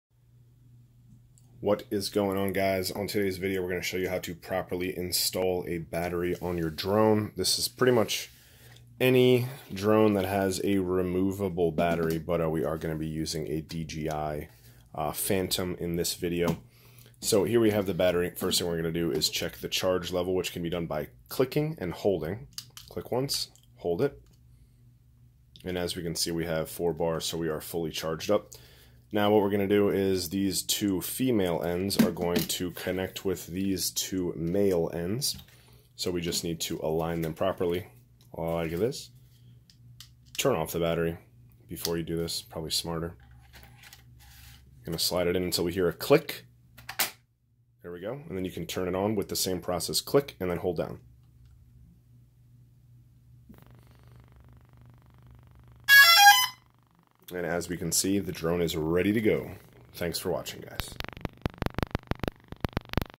How to Install a Drone Battery
In this video I will show you how to install a battery in your drone.  We use a DJI Phantom for this video, however the process is the same for most drones.